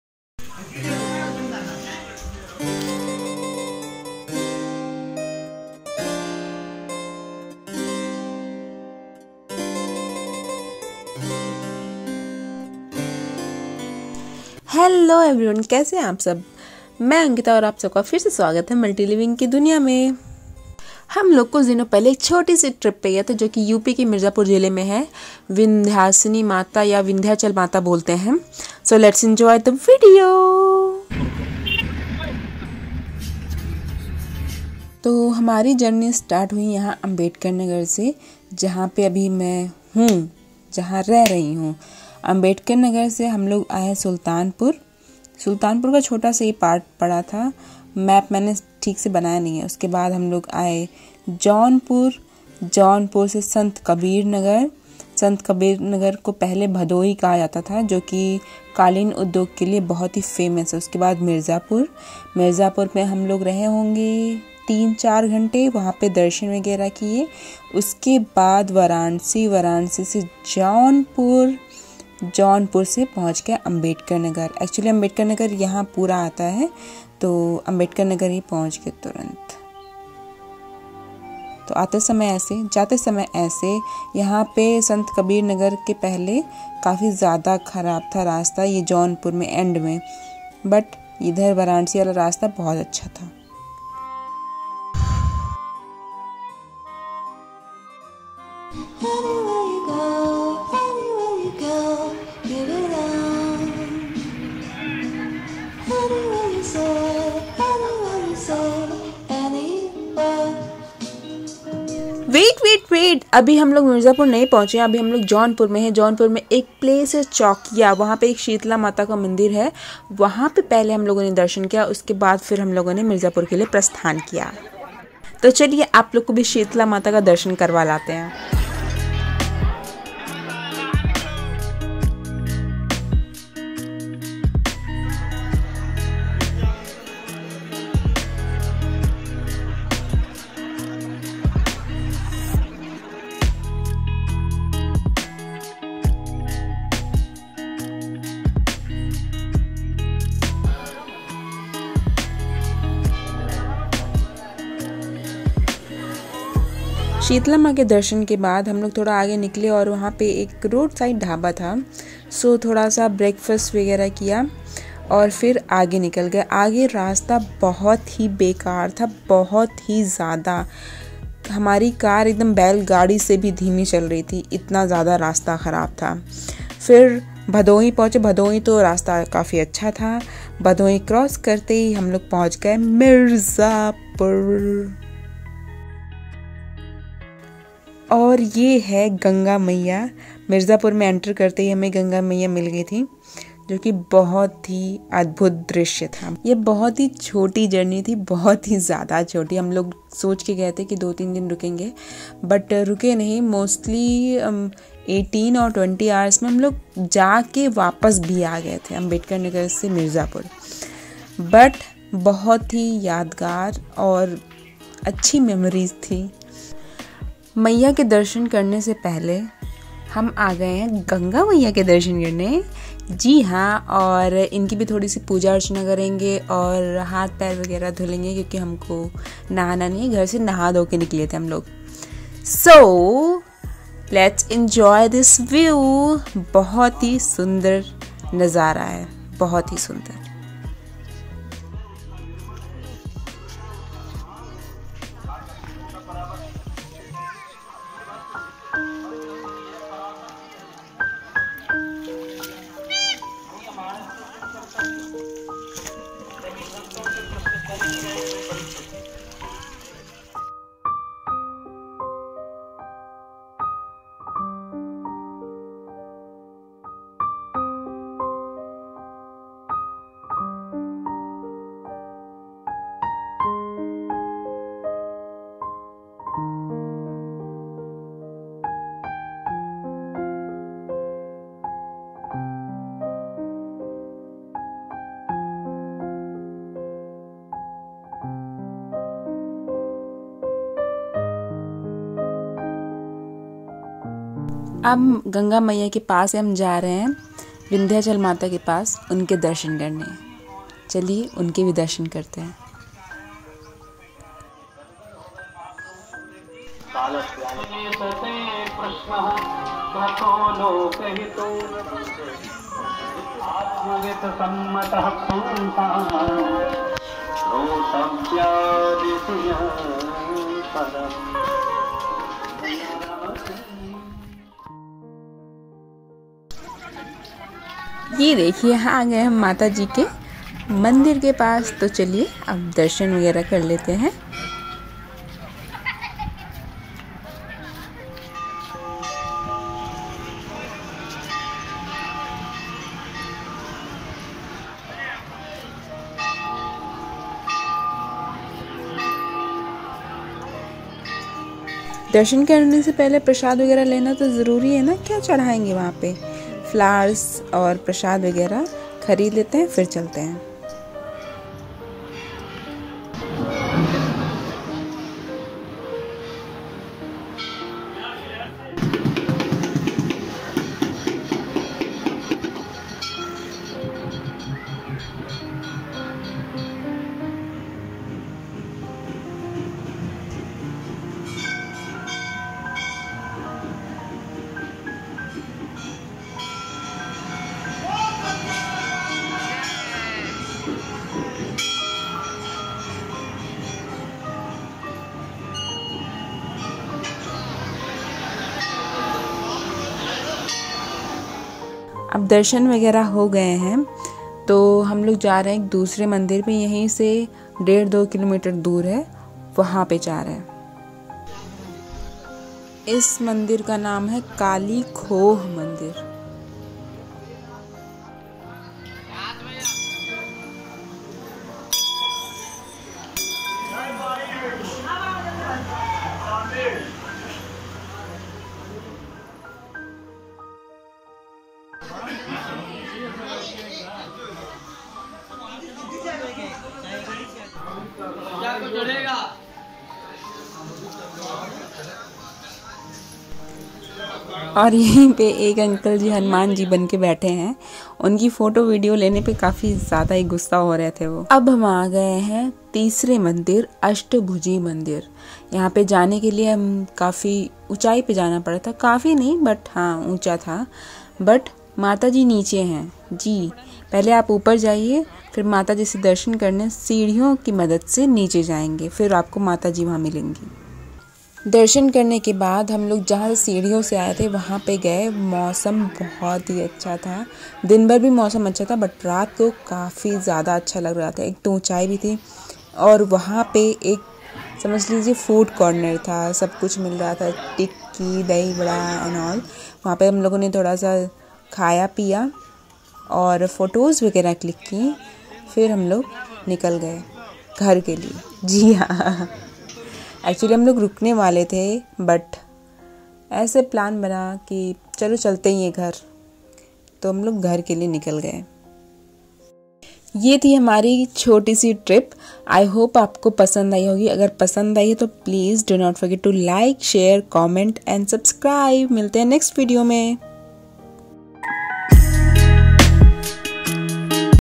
Vindhyachal Vlog | A small trip to Vindhyachal devi Mandir in Mirzapur | Family outing | travel vlog
Hi everyone! I'm Ankita, welcome back in the world of Multi Living.